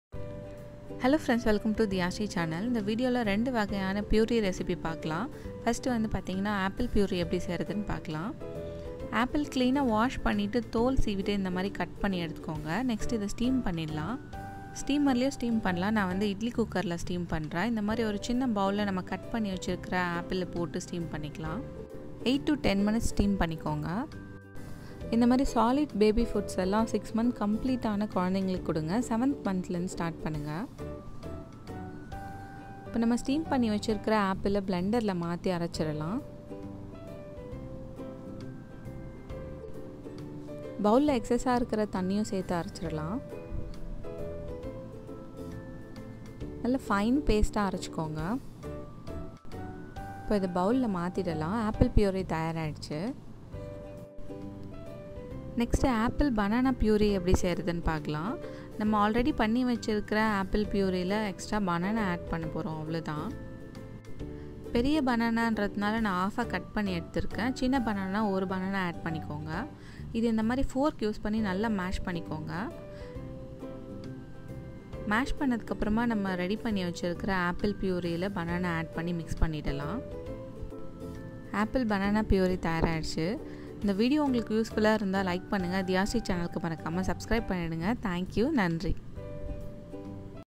Baby food recipe | apple purée | apple banana purée | 6m plus baby foods | healthy baby foods
Hello friends,
In this video I explained about how to make baby(6 month +) food recipe in tamil

Ingredients
Apple
Banana